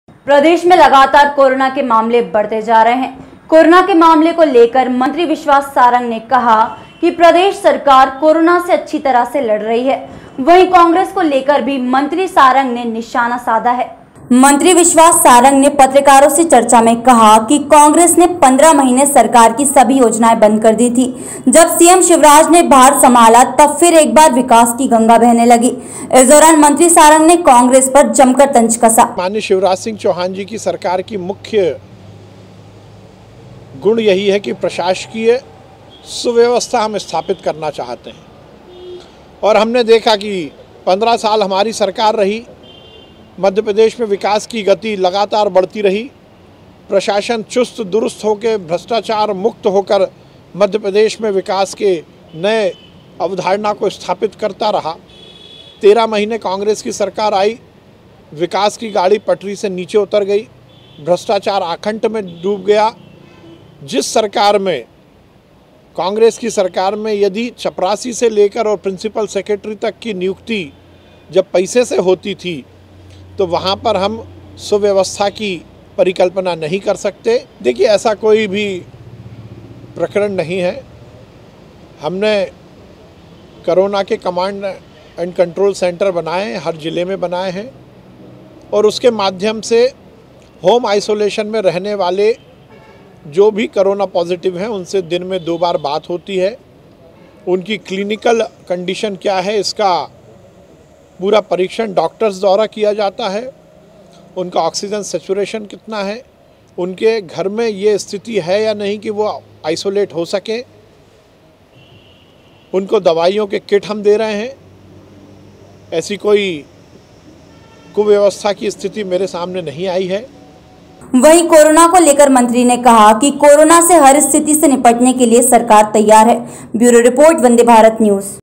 मंत्री सारंग का कांग्रेस पर तंज, सीएम शिवराज की जमकर की तारीफ। Vande Bharat News
प्रदेश में लगातार कोरोना के मामले बढ़ते जा रहे हैं। कोरोना के मामले को लेकर मंत्री विश्वास सारंग ने कहा कि प्रदेश सरकार कोरोना से अच्छी तरह से लड़ रही है। वहीं कांग्रेस को लेकर भी मंत्री सारंग ने निशाना साधा है।